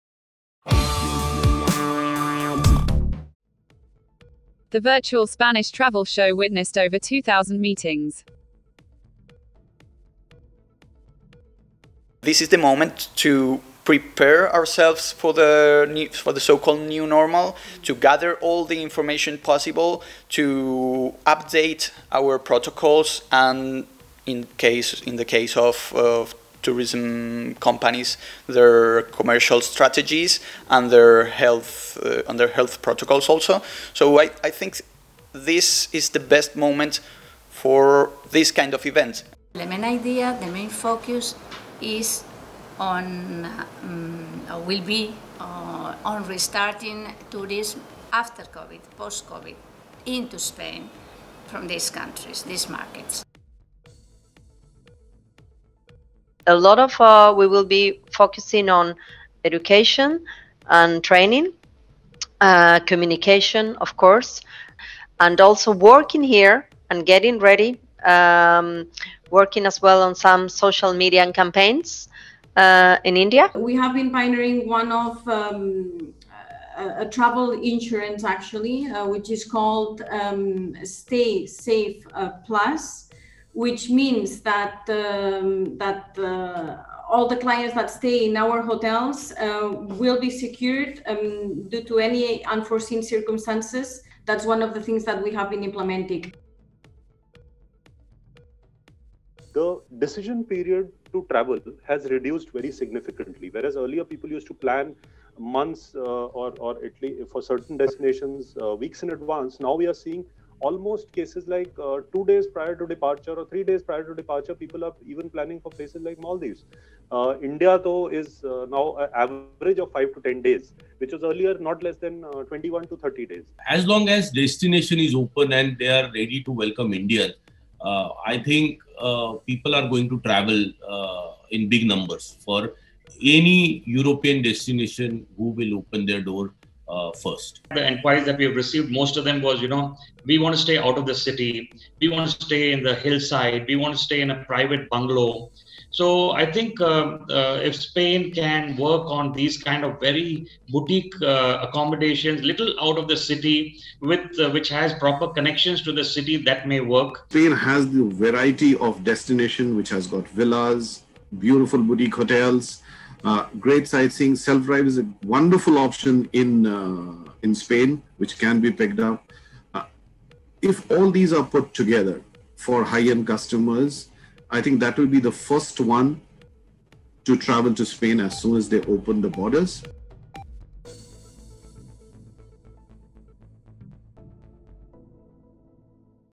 Traveltv.news Episode 1250 (09.12.2020)
In this episode TravelTV.News focuses on:
The Virtual Spanish Travel Show witnessed over 2000 meetings
We Interviewed
RAUL DE LA ROSA MACHADO, Counsellor, Embassy of Spain in India
ELISA ROBLES-FRAGA, Director, Tourism Office of Spain in India
BELEN PEREZ GASCON, Business Development Manager, Emerging Markets & Air Connectivity, Visit Costa Del Sol
SANDRA POLO CANUDAS,  Asia & ME Commercial Director, Palladium Hotel Group
ROMIL PANT, Senior Vice President-Leisure Travel, Thomas Cook (India)
HIMANSHU PATIL, Director, Kesari Tours
MANISH KRIPLANI, Managing Director, Baywatch Travel
DEEPAK RAWAT, Senior Vice President International Markets, MakeMyTrip